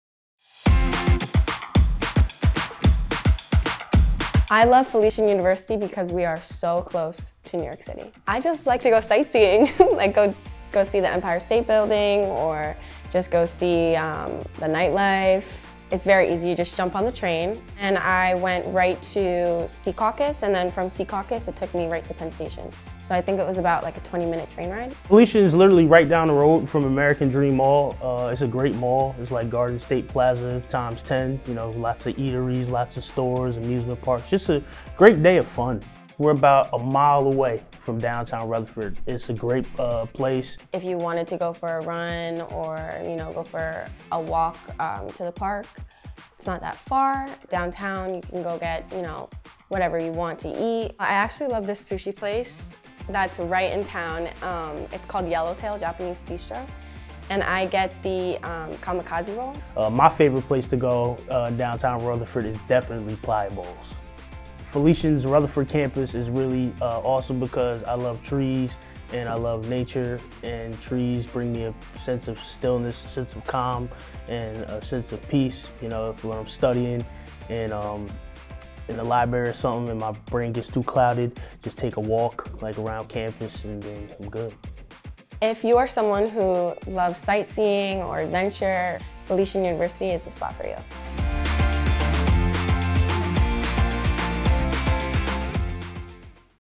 Around Felician | Felician University
Welcome to Rutherford, New Jersey, home to Felician and all our Golden Falcons. Located just 10 miles away from New York City, this town known as the Borough of Trees has it all: restaurants, shops, parks, entertainment, and more.

Plus, you can get to NYC in just 30 minutes or less by train or bus, with stations just a mile away from Felician. Discover why not only our students but people from all over love Rutherford!